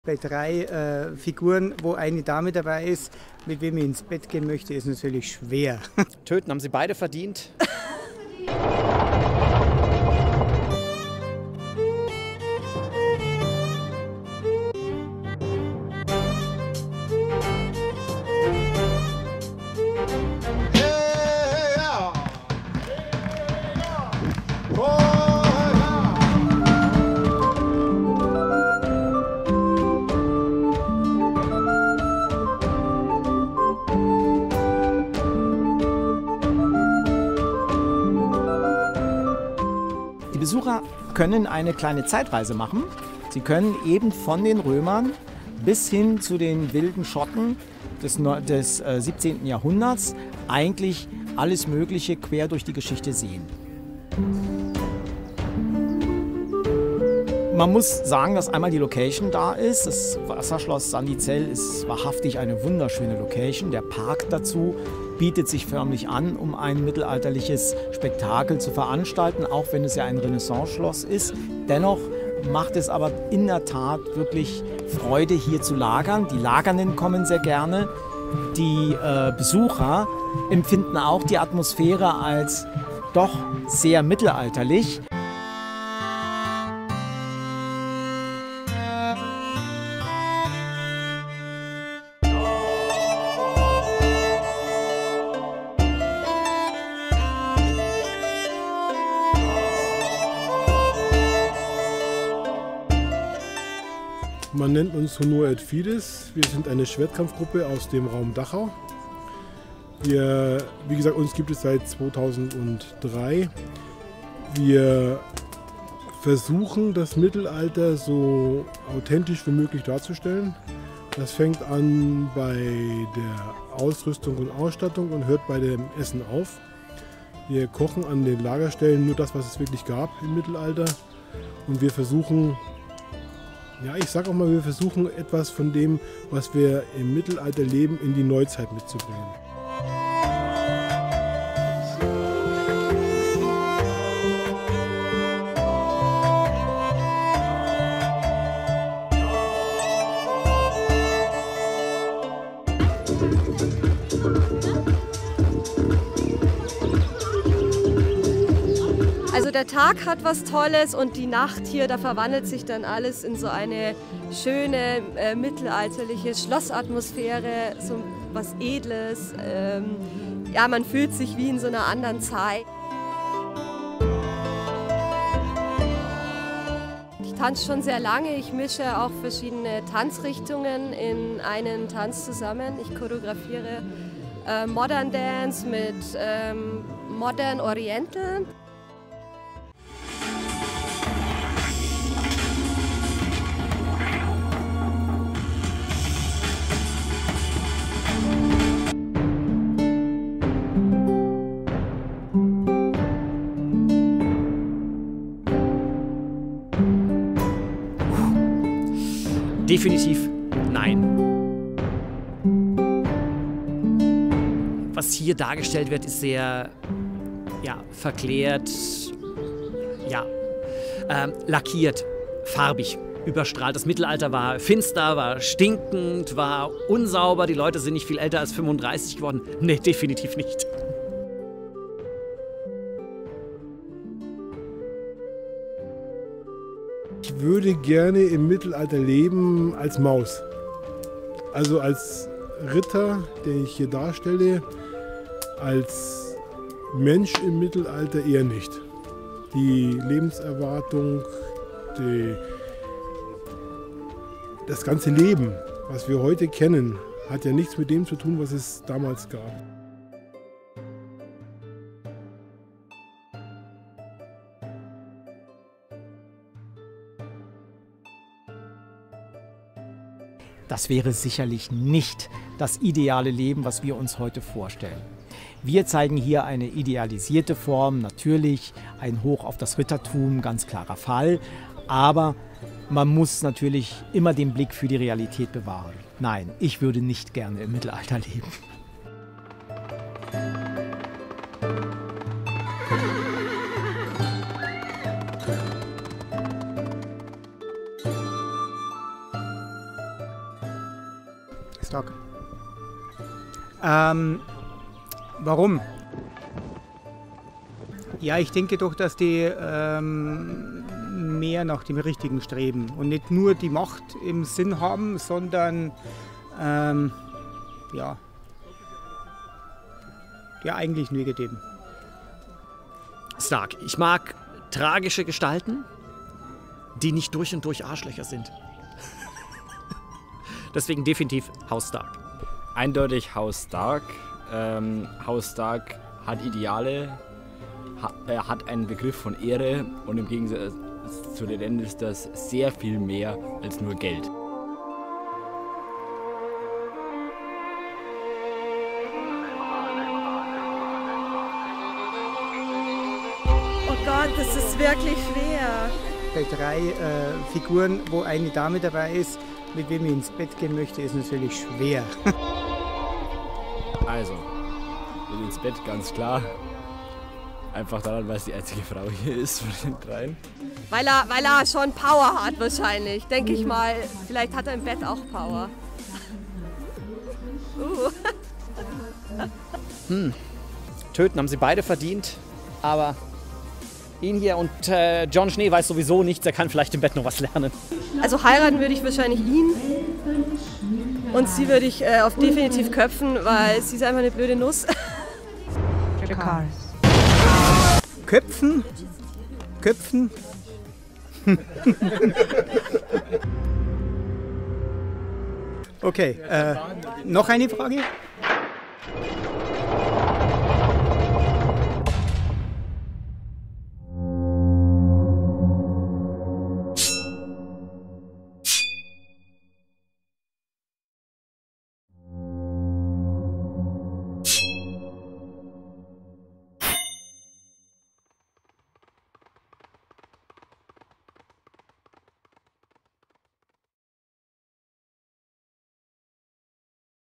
Mittelalter Spektakel Sandizell 2019 / GAME OF THRONES - Stark oder Lannister?
Eingefleischte Mittelalter- und Game of Thrones-Fans kennen diese Zeit nur allzu gut – das Mittelalter.
Wollt ihr selbst auch einmal für eine kurze Zeit in der Welt von Haus Stark, Targaryen und wilden Drachen leben?

Ein ganz besonderes Fest in der Nähe von Ingolstadt machte dies vom 20. Bis 23. Juni 2019 möglich!
Mit dem mittelalterlichen Spektakel auf Wasserschloss Sandizell wird ein jeder Besucher verzaubert und kann in die damalige Welt der edlen Damen und mutigen Rittern im Mittelalter eintauchen.

Kamera und Postproduction:
Ser Daniel Witschkanow

Interview Leitung:
Lady Christina Binder

Konzeption und Planung:
Ser Daniel Witschkanow
Lady Christina Binder

Verdankung:
Lord Walter Mehl